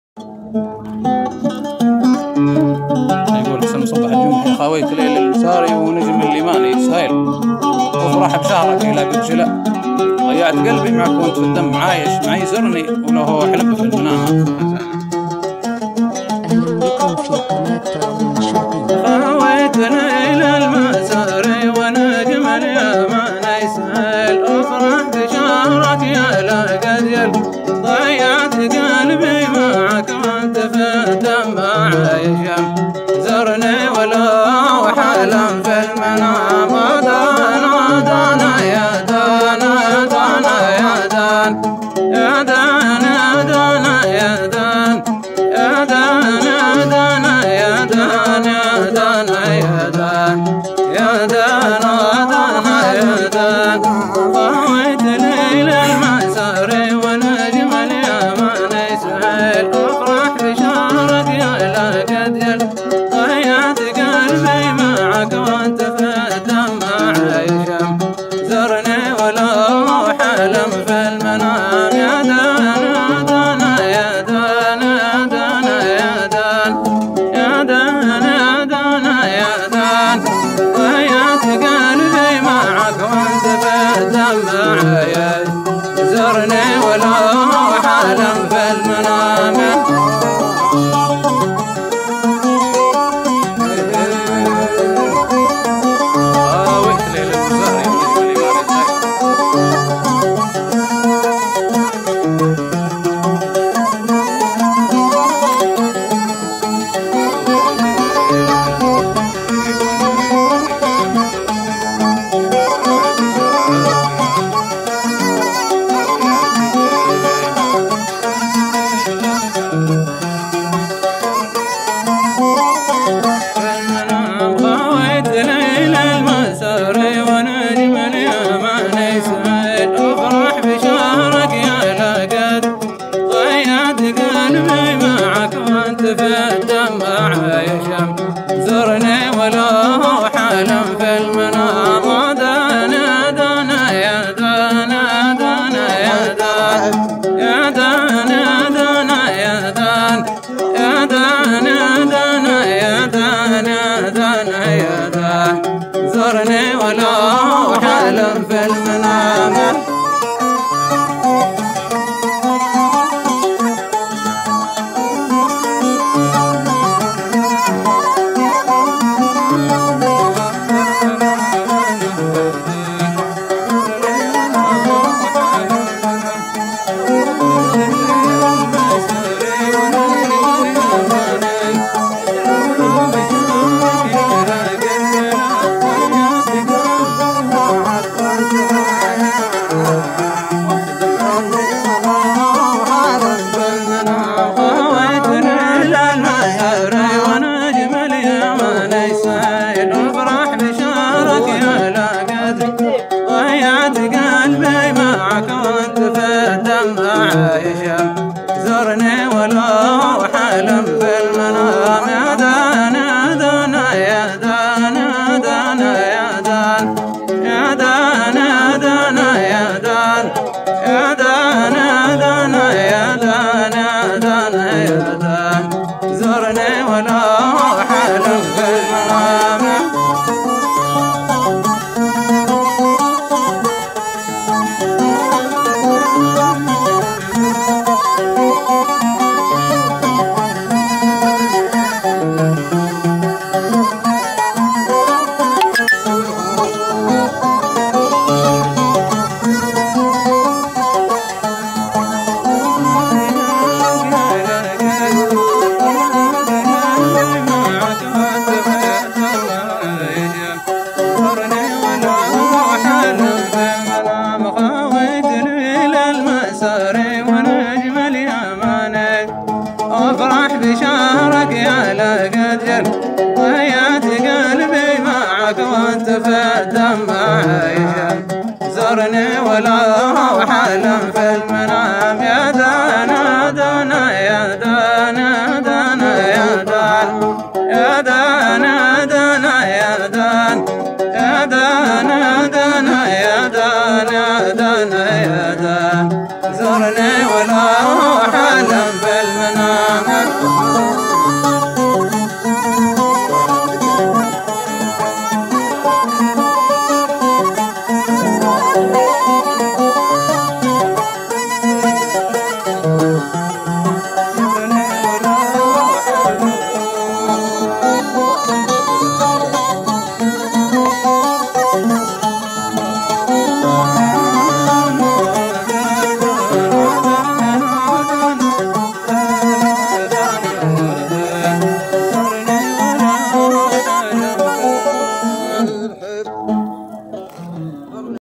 صوت دان شحيب ابو قصي وابوقنبله وحمد مبارك وسالم بن شطيف جلست سمر زواج بيت عسانه في المصينعه
جلسة سمر دان شحيب في بيت الشيخ دليل العساني بمناسبة ..زواج ابنه سعيد دليل في منطقة المصينعه ـ الريدة الشرقية .. السبت 28/12/2021

💲اهداء خاص للعريس سعيد دليل العساني والف مبرووووك الزواج 💲

صوت /ابوقصي
حمد مبارك الشنيني
ابوقنبله الشنيني
سالم بن شطيفه
عازف عود /خالد صرخه
مزمار /احمد بن عبيدان
الصوت من الحان /ابوقصي باعباد
وكلمات الصوت /للشاعر 1 حسن طيور باعباد

ياليل ع شأن عربان يستاهلون الجميل
مادي مع عاشقين السلأ
الجبر والواجب وجميل خلاني ماينتسي
والارض فيها يطيب المقام
2
حسن مصبح الجمحي

خريت وديان عشوه على شانكم يادليل
ضاعن على غيثكم لي دلأ
ظهر المطيه وقلبي من الشوق ماهو خلي
ماضي وحاضر عليكم سلام

3
حسن طيور باعباد

 بيني وبينك مروات من كالها يستكيل
ومعبرين الوفاء بالوفاء
الشوق ضوى زهابي ومركوبي الاولي
والعاطفه والموده خطام

4
حسن مصبح

الله يرعى عهود اللقاء والزمان الدويل
ياوقت رجع لنا ما مضا
فارقت عربان ساروا وخلوني في حيرتي
من بعدهم نا وقلبي يتام

5
ابوعلي السيفي

تقديركم وافي ومقامه الغالي دايم ثقيل
والجود صرفه يرد الجزاء
والجبر في الدنيا وكلما خلق فاني ياصاحبي
مهما تطول الشهر والعوام

6
محمد بن الشرف

ياخير ليله بدأ نورها مالها شي مثيل
وتوافدت ناس للملتقى
لا با وصف ناس بالجود ماكذبت ياهاجسي
باغلق الحبر عاده كلام

7
حسن طيور

قلبي قسمته ووديت به للمحب والخليل
عدى زماني عناء في عناء
من حيت ماهل طاريك يالشامخ المعتلي
من غير حسي ضرب لك سلام

8
حسن مصبح

خاويت ليل المساري ونجم اليماني سهيل
وافرح بشهرك ايلا قد جلأ
ضيعت قلبي معك وانت في الدم عايش معي
زرني ولو هو حلم في المنام

9
محمد بن رهيم

منشأ الحميمه من الشرق خلى القهيله تسيل
وفروع سالت عليها ولأ
ياطايف النوب حصل مباقيل ماتشتني
 الغيث روح عليها قسام

10

حسن مصبح

يابن عوادي حنين العوادي رقلني رقيل
لكننا مامعي شي ولاء
رديت من فوق جبحه  وكفي وصيني خلي
خايف من اللوم هو الحرام

11
حسن طيور

من حيت ما مالت العين قلب البنادم يميل
سر الهوى بين اهل الهوى
الشوق حرك مشاطين في خاطر المبتلي
مرت عليها ثلاثين عام

12
ابوعلي السيفي

من شد سلطانه وحل وديان ضيف ونزيل
الارض خضراء وفيها جناء
ومخال ع القبله وحيت ماسبل دايم قوي
وصبحت قصور المحبه حطام

13
حسن طيور

ليل الشتاء يالمحبين ماشوف موحه طويل
معاد شي فالليالي مدى
والقافله حيت تعتاد يا حاجز البندري
مصباحها عند مولى شبام

14

محمد بن الشرف

ياعاشق النوب بيّت للشوق مثل الدخيل
من شهدها بايجيب الدواء
ياكم دباس قصرت حباله وعاده مدي
رسى بها في نواف الخشام

15
حسن مصبح

يارب ماذل من هو وقف فوق بابك دخيل
ماباتخيّب لعبدك رجاء
كُرمه لموسى من الصخر فجرت مورد هني
يالله بشربه نهار الزحام

16
حسن مصبح

الريح لي هب طيّر علينا خريف النخيل
لكنهن رووسهن فالسماء
لي فات يتعوض مادامه الجذر والرأس حي
ماباقنع بالكرب والقصام

17
حسن مصبح

بين المحبين ظلم الجفاء والقنع مستحيل
لاتخلنا بين قاع وسماء
ياشوق وديت نا وياك ع بر آمن رسي
قبل السفينه تروح حطام

18
ابوعلي السيفي

باقاسم الجود ناعاد شي فالذخاير قليل
ولا قفلنا بواب العطاء
ياشاكي الوقت كمين مظلوم ع يشتكي
بينه وبين السعاده ظلام

19
حسن طيور

من كُرم ربي اذا راد كل شعب يضوي بسيل
ويغيث وادي حلوله ضمأ
رزق البريه على خالق الكون يابوعلي
والفانيه زام من بعد زام

20
حسن مصبح

مالت عليك السبوله ونا قسمتي فالصميل
ماهو سواء الحظ ماهو سواء
منصور هادي كياسه ملآنه وراسه خلي
الرزق ماهو بكثر الفهام

21
محمد بن رهيم

من هبة الماضي راحت مقاصيد كم من عميل
والسلسله عاد فيها مدى
لي هو توكل تحمل ولو كان رأسه بري
شل المظالم وحق اليتام

22
حسن مصبح

وادي بلس في بحور المواج الغزيره يسيل
هنوه لمن هو شرب واستقى
من غير اهل المضابي جميع العرب تستقي
واهل المضابي عليهم حرام

23

حسن طيور

ماشوف شي في هوى الزين غالي ولا مستحيل
لو كان ربي عطاني ولاء
ناباتحمل خطاياه وان كان شافه شوي
باسامحه فالصلاه والصيام

24
حسن طيور

ياليل قلبي مخاويك ورميت به لسبيل
ع هوين ماحد يلقّي كذا
الناس قالوا من العام ماحد يخاوي غثي
لكنني ماسمعت الكلام

25
حسن مصبح

يالجبر ياريت ساير زمانك يقع فالسليل
ماباحسب ع صباح ومساء
كل من تحبه تمناه لما قده يكتفي
لوجيت بين الريش والقسام

26

محمد بن الشرف

ياقلب فالعشق مالك مخاوي ولا لك دليل
وبقيت بين السهن والرجاء
الله يعين الموالف من الشوق ماهو سلي
ومن الضما ع يصدق غيام

27

محمد بن الشرف

بعض الجرب زينه تلقي محاصيل دايم تكيل
حبيتها ع خصيب الثرى
وان كان رعدت على الفرع كل سيل ماضر شي
حيت المجارف عليها سوام

28

حسن مصبح

غاليت في السعر ولقط لي عيلماني طويل
تعرف قروحه جميع الزياء
مالوم من توّل مصروفي الزين وسلوبتي
الحسد رأس البلأ والخصام

29

حسن طيور

الوقت ع غير ميزان لاعدليته يميل
ومطارد الشارده في الخلأ
مابايضوي عسل من شناطيب ماتلتفي
والجوهره من على رأس هام

30

حسن طيور

شي بدليته وشي لا تحسيت مابه بديل
قلب البنادم على مانوى
ان كان شي دار ع خاطرك حب ولا رثي
باب الجفاء والقطيعه حرام

31

ابوعلي السيفي
مادامه الوقت ياصاحبي فيه قايل وقيل
ايش بايقوم سواس البناء
ذكرك على بالي محبوب غالي ولا تشتني
مابانخالف خطوط النظام

32

حسن مصبح

قوس الطمع جاب كم من مصّوب وكم من قتيل
يابخت لي ماعُقُل ودرى
والرأس لي هو  فـدورة عقاله كُبر مايجي
ياصاحبي خير منه الغرام

33

بخيت العدلي

ياليل جمعت كل الخواطر فواجب دليل
لكن فرقتهم في الحكى
لا باتبع صاحب القلب لي فالمحبه بلي
باعطيه ع كل كلمه كلام

34

محمد بن الشرف

ياوقت توّلت حتى الذهب لي عياره ثقيل
والقيت بيني وبينك برى
ياوافي الكلمه معروف ماحد يكذب وفي
ع كلامكم بانحط البهام

جمع وتدوين ابوقصي باعباد
مع خالص تحياتي ✋🏻